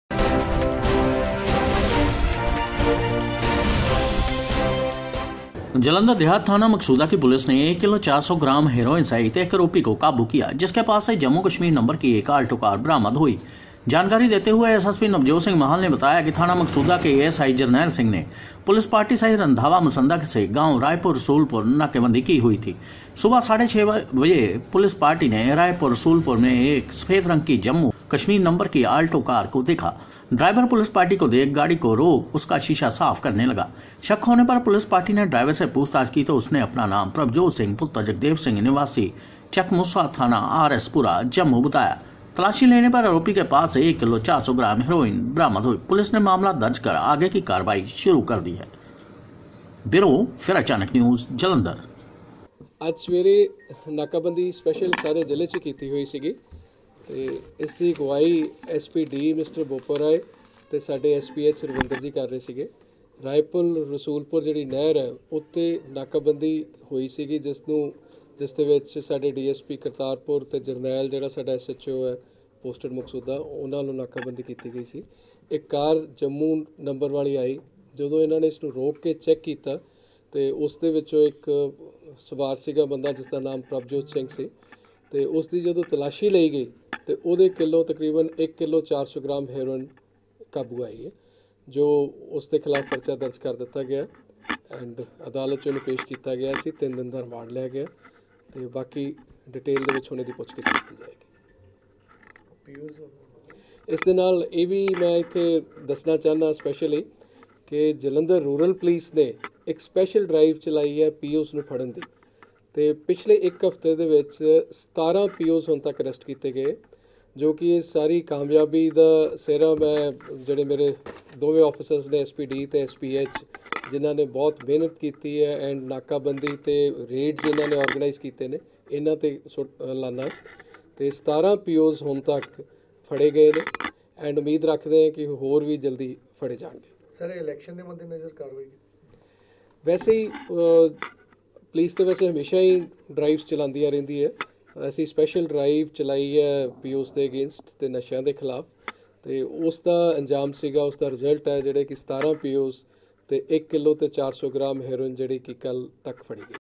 जालंधर देहात थाना मकसूदां की पुलिस ने 1 किलो 400 ग्राम हेरोइन सहित एक आरोपी को काबू किया
Video from Paramjeet Singh